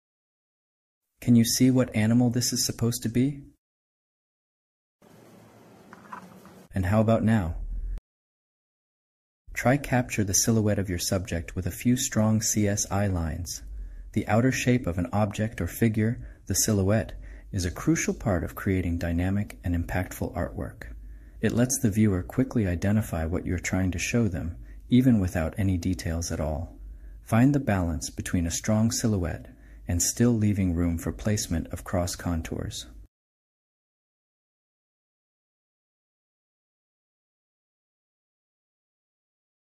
make a clear silhouette when drawing and painting
make sure the silhouette of your subject is clear and easy to see.